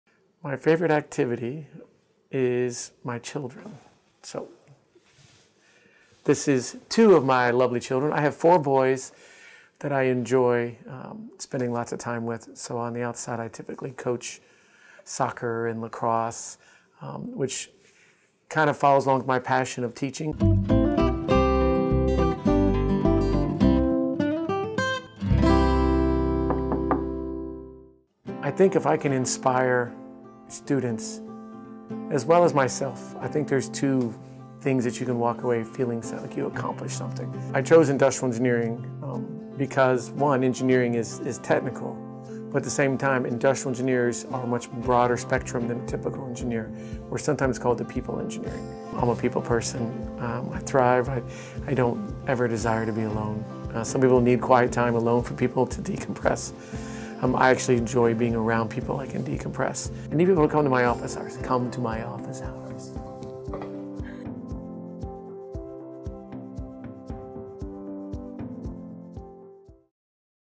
Office Hours: USD Associate Professor of Industrial and Systems Engineering, Leonard Perry, PhD
Casual conversations with USD faculty addressing interesting facts about them, their personal interests, their work and their research. Meet Dr. Leonard Perry - Associate Professor of Industrial and Systems Engineering at the Shiley-Marcos School of Engineering.